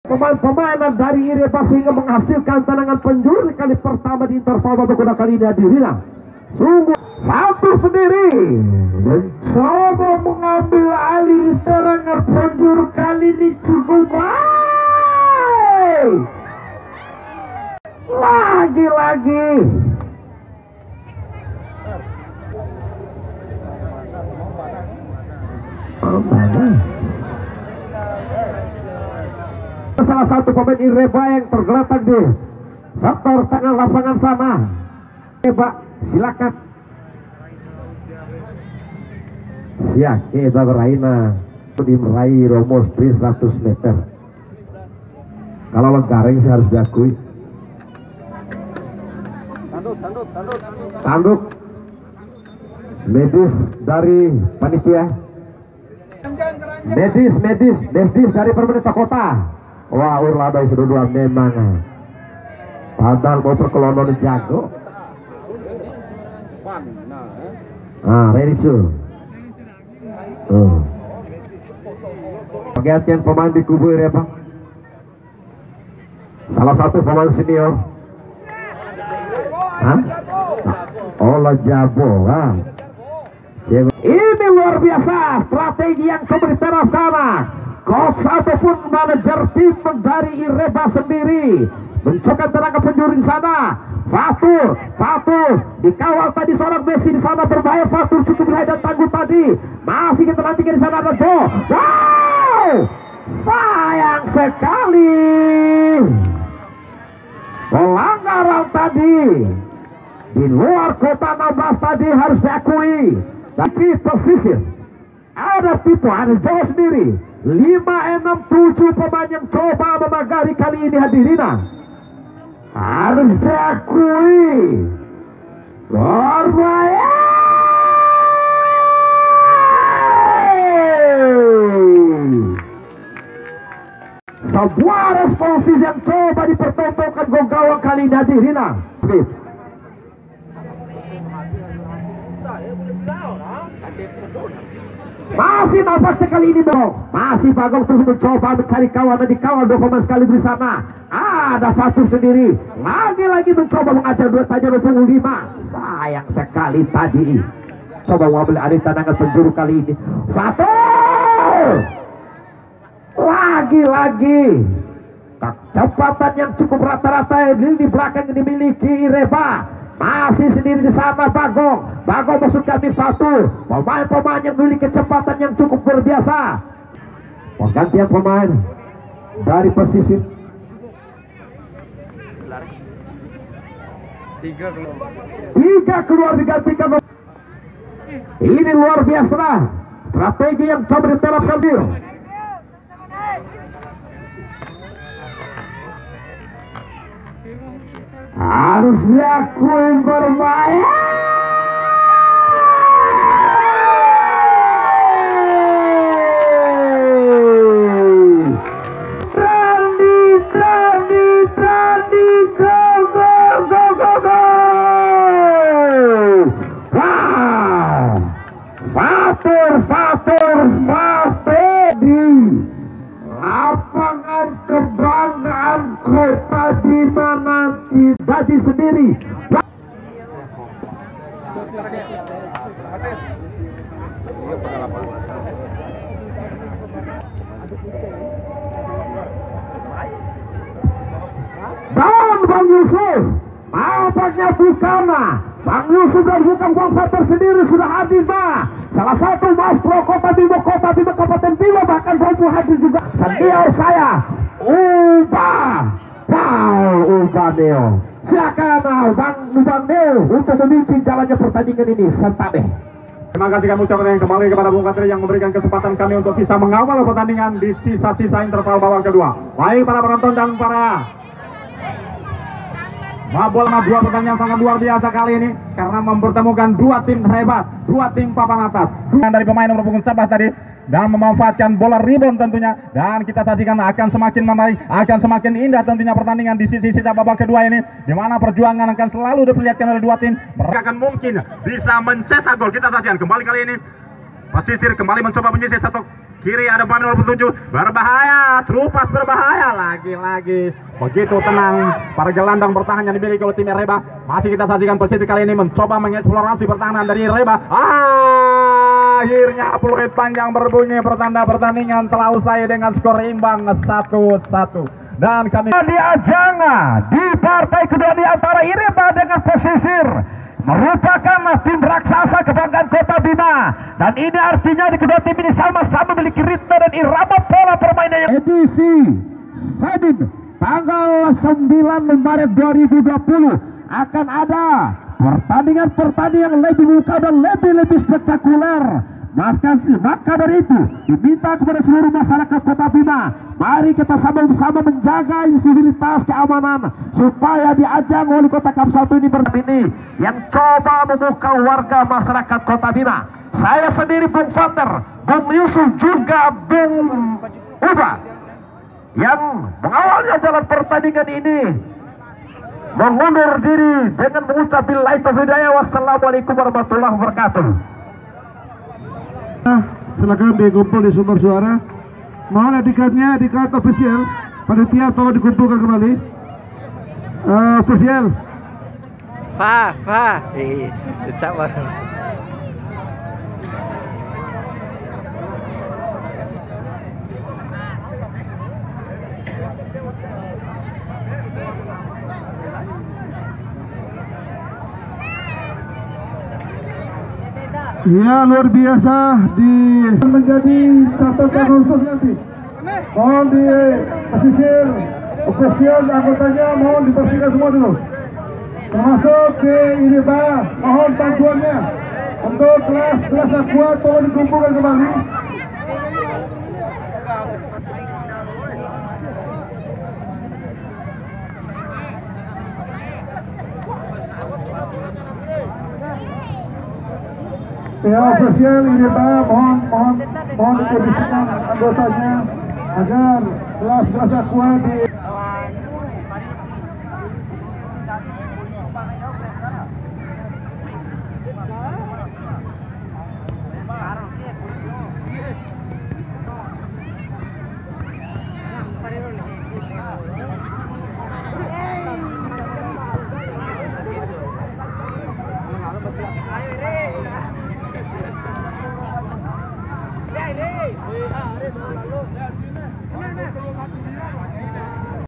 Bagian 5/5 Piala Walikota Cup Kota Bima Gol Sekali sentuhan Ireba (Gilipanda) Vs Pesisir (Melayu)
Perdana. Langsung di liput oleh Chanel Youtube Dae san, biar tidak ketinggalan vidio terbaru mohon dukungannya dengan cara subscribe (Tekan tanda lonceng), like and komen. Semoga Di Piala Walikota Cup Kota Bima ini melahirkan bibit baru Bagi Persepakbolaan Kota Bima (Persekobi) sehingga para panitia pelaksana maupun para pencari bakat bisa menemukan talenta-talenta berbakat